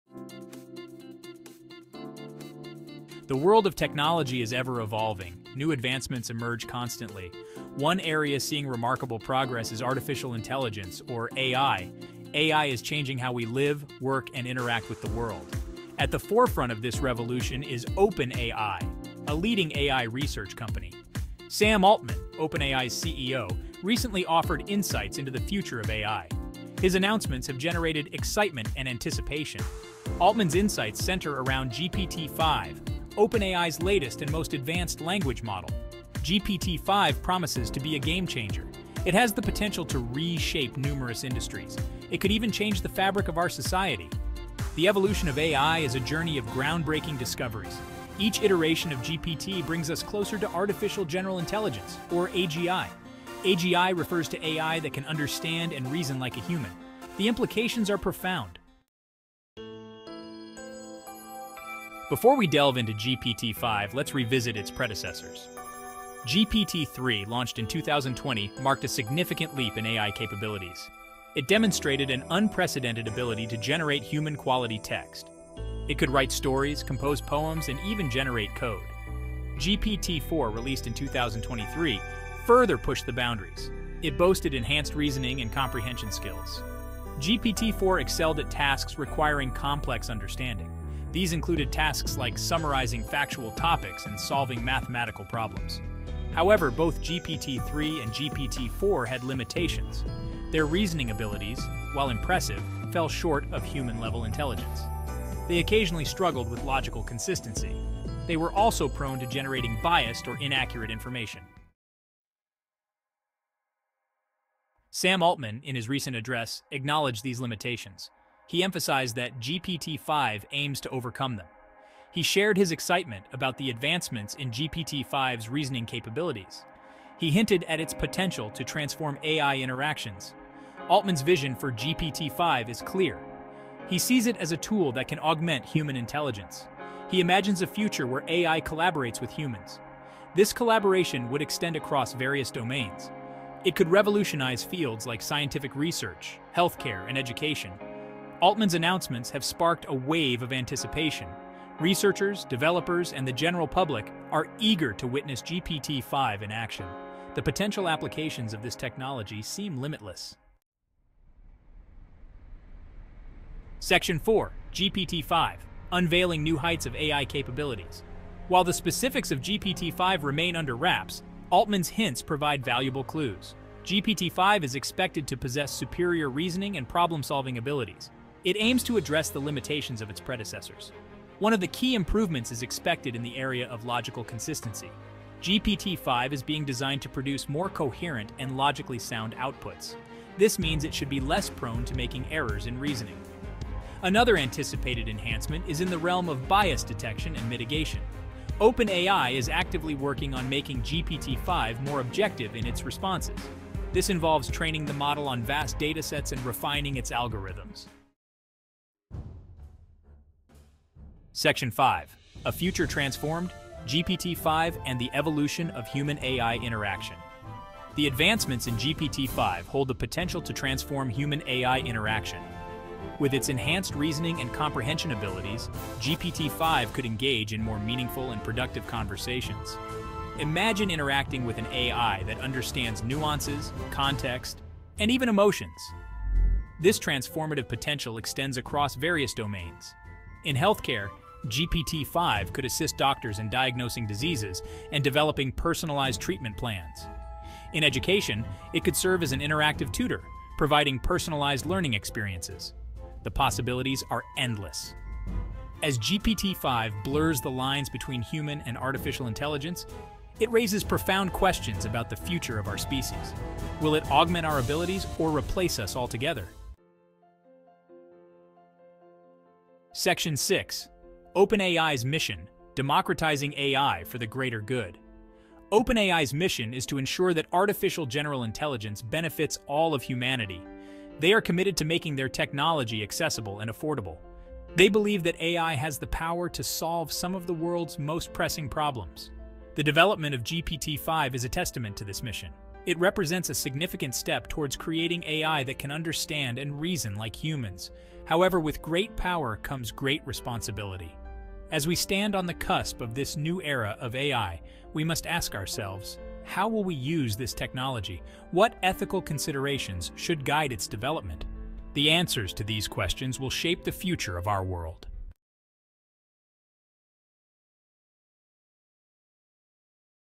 Sam Altman Reveals Breakthrough with GPT-5: A Major Leap in AI Technology
**Preparing for the AI Leap: OpenAI's GPT-5 Stepping up the Game**

OpenAI's CEO Sam Altman has provided intriguing snippets of information about the development of the highly-anticipated Generative Pre-training Transformer 5 (GPT-5) - an upgrade he characterizes as a significant leap forward from previous models.

ChatGPT, launched in November 2022 by OpenAI, marked a true watershed for AI technology, promoting OpenAI and Altman into the limelight overnight. Now, Altman has unveiled plans for the next Language Learning Model - the GPT-5. Speaking at the Aspen Ideas Festival, he shared his optimism about the future model, however, admits there is a lot of work yet to be done.

Despite the developmental challenges, Altman is confident about the new AI's potential. He pinpoints its objective of addressing and drastically reducing errors observed in the previous model, GPT-4. However, he maintained a cautious optimism, acknowledging the realities of perfecting such a complex development: "I expect it to be a significant leap forward. A lot of the things that GPT-4 gets wrong...we still have a lot of work to do on it."

Interestingly, the revealing of GPT-5 comes closely after the release of the GPT-4o model, which has been praised for its abilities to solve equations and present conversations with high emotional competency.

As we await further details and the launch date, one question looms large: What significant advancements are set to revolutionize our experience with AI models, and how will GPT-5 redefine our interactions and understandings of AI capabilities?

Discover more advanced AI tools at [Murf AI] - enabling real voices to be created with AI.

 Sam Altman Drops Hints About GPT-5 by Steven's Workspace

OUTLINE:

00:00:00 The Unfolding AI Revolution
00:00:58 Milestones in AI Development
00:01:51 A Glimpse into GPT-5
00:02:40 Unveiling New Heights of AI Capabilities
00:03:28 GPT-5 and the Evolution of Human-AI Interaction
00:04:26 Democratizing AI for the Greater Good